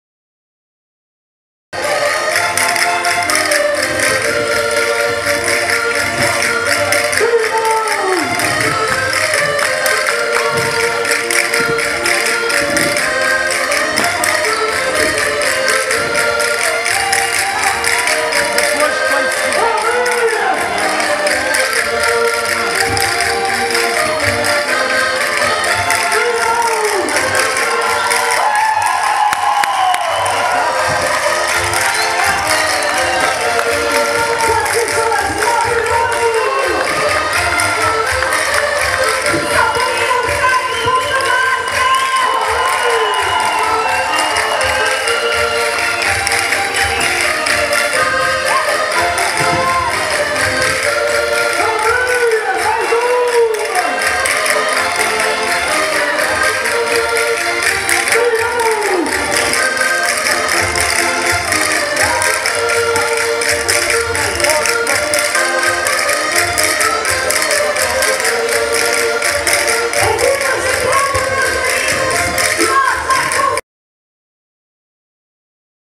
700 Pessoas a Dançar ao Mesmo Tempo o Vira! Ponte da Barca
Todos os vídeos são de Nossa  Autoria!

paisagens, festas, romarias, cantares ao desafio, folclore...